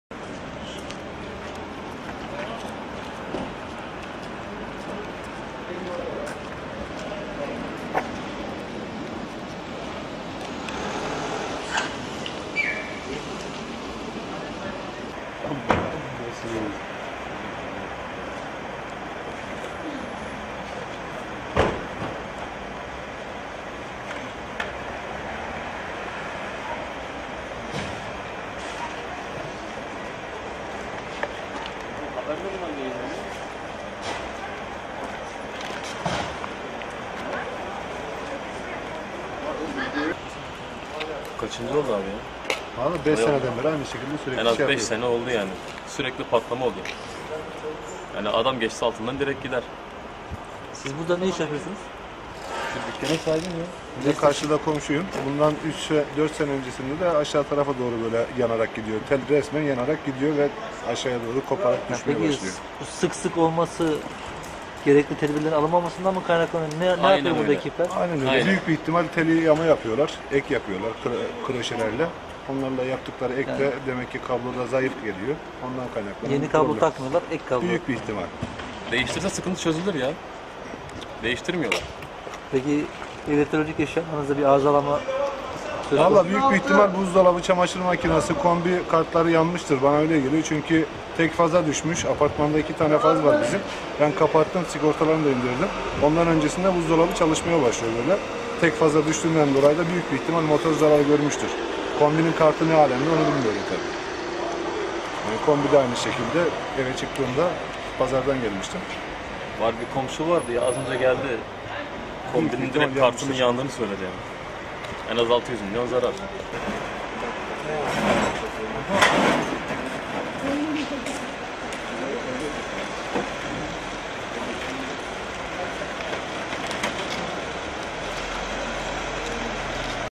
Artık kopmaz!
Son bir ay içerisinde üç kez kopan elektrik teli mahalleyi ayaklandırınca daha kalınını taktılar. TREDAŞ arıza ekipleri, “Merak etmeyin artık kopmaz.” dedi!